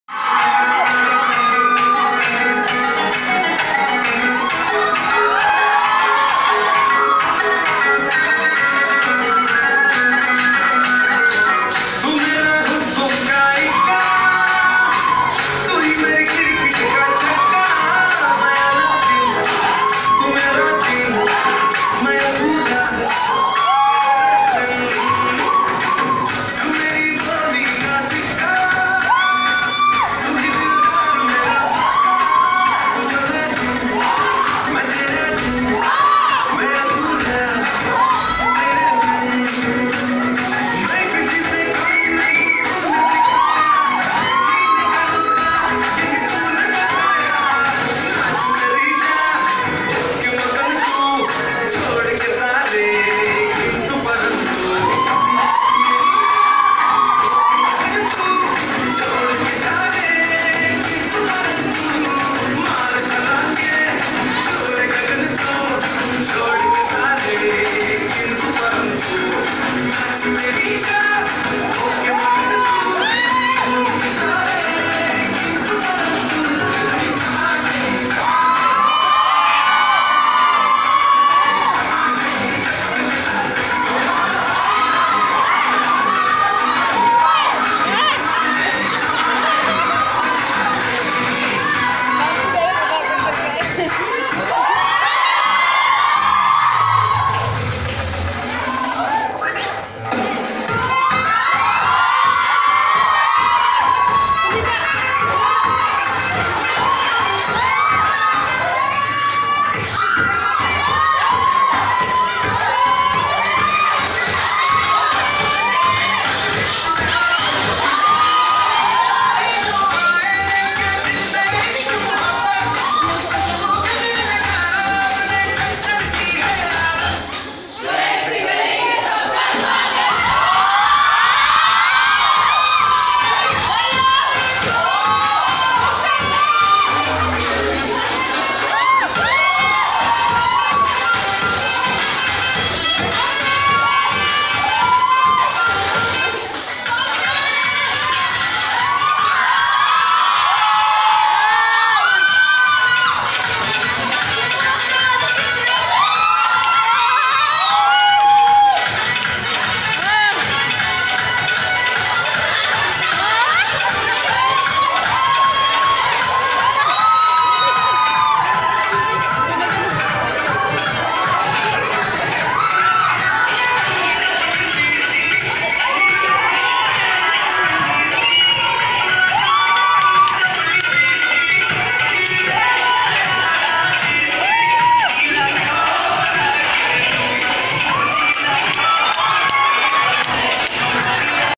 Tomo Riba state nursing school student dance(TRIMS), naharlagun during farewell celebration 2018 at
Nurse party